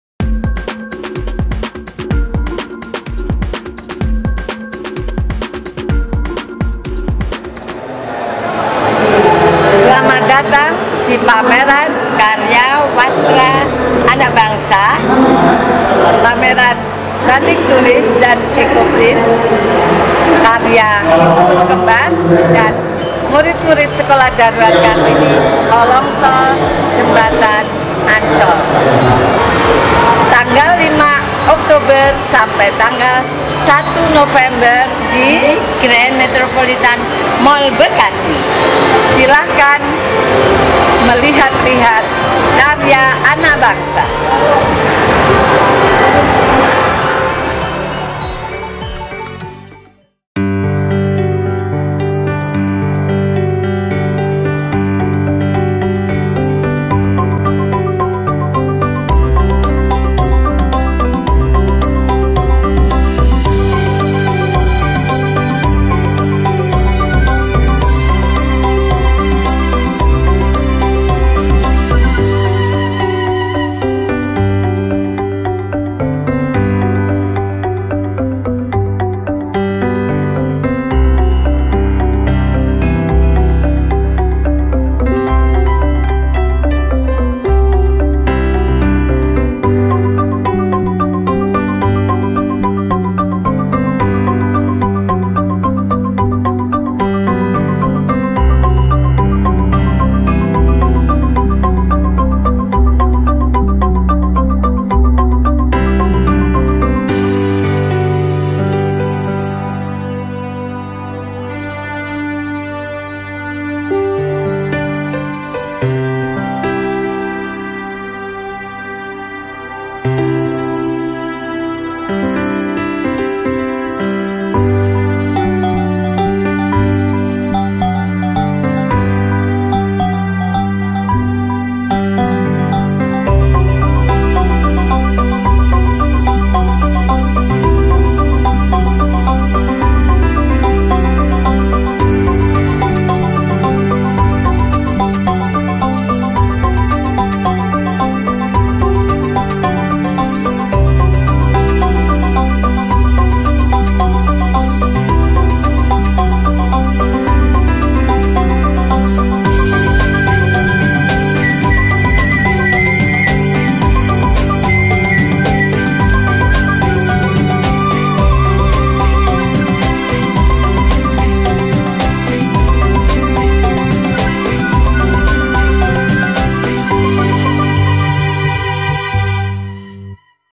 Pameran Kain Batik Karya Murid Sekolah Darurat Kartini Asuhan Ibu Guru Kembar
Sekolah Darurat Kartini Ibu Guru Kembar bersama Yayasan Metropolitan Peduli menyelenggarakan Pameran Karya Wastra Anak Bangsa bertempat di Grand Metropolitan Mall Bekasi.

Acara diselenggarakan 5 Oktober hingga 1 November 2020. Pameran ini menampilkan Kain Batik, Ecoprint dan Rajutan Karya Ibu Guru Kembar dan Murid-Muridnya.

Sebagian besar karya yang dipamerkan adalah hasil buatan selama masa pandemi covid-19 ini.